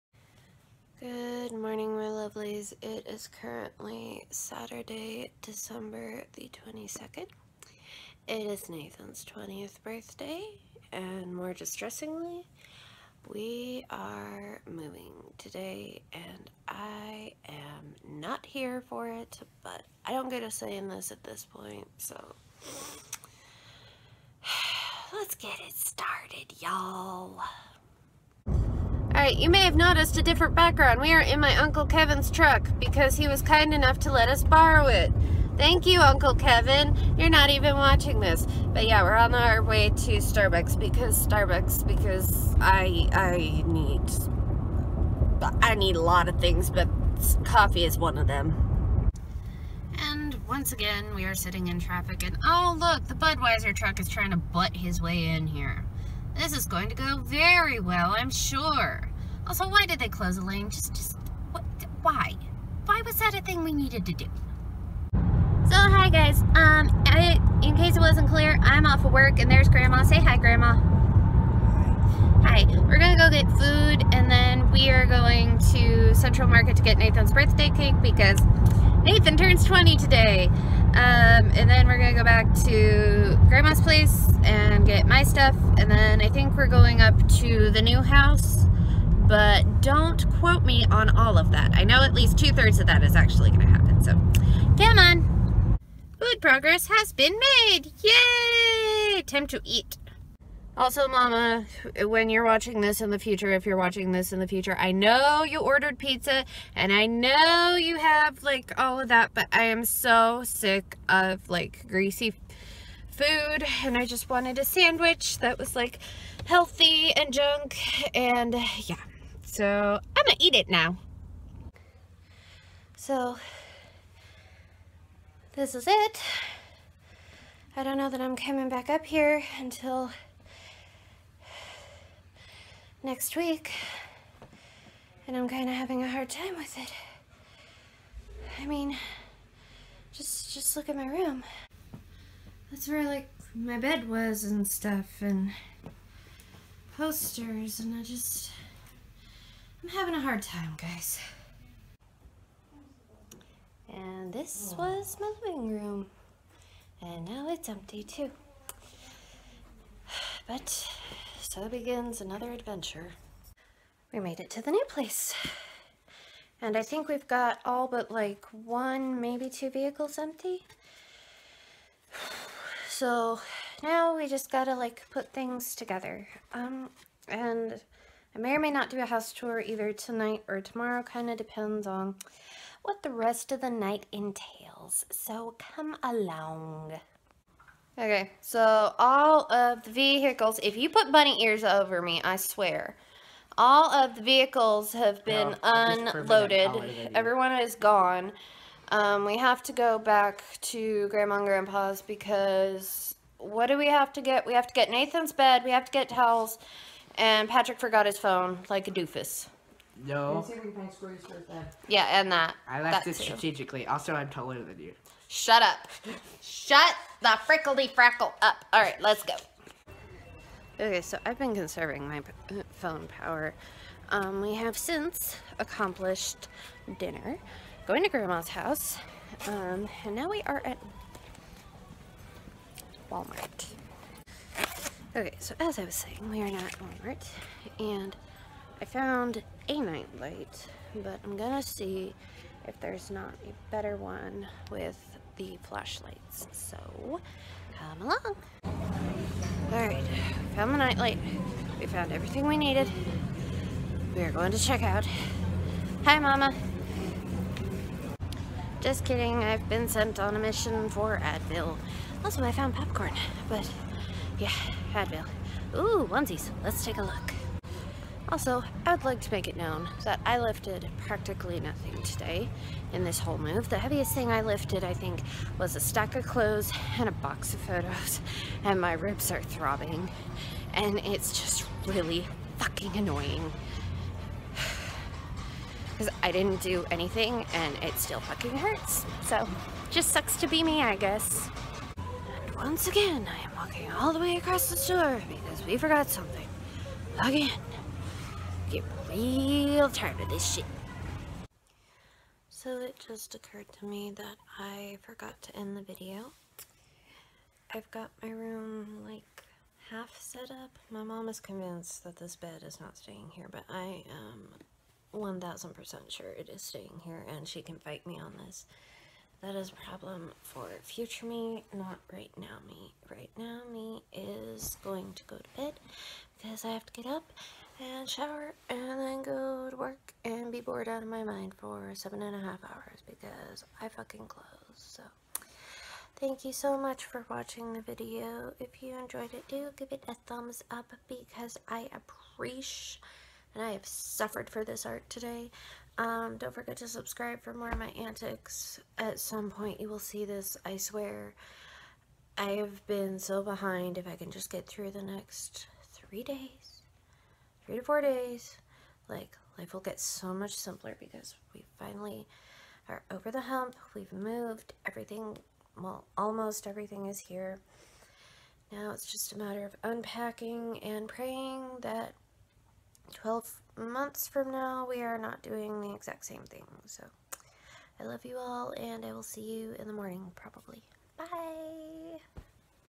WE DONE DID MOVE (12/22)
Come along as I go to work, have a minor crisis over my empty house, move to a new one, and more! :D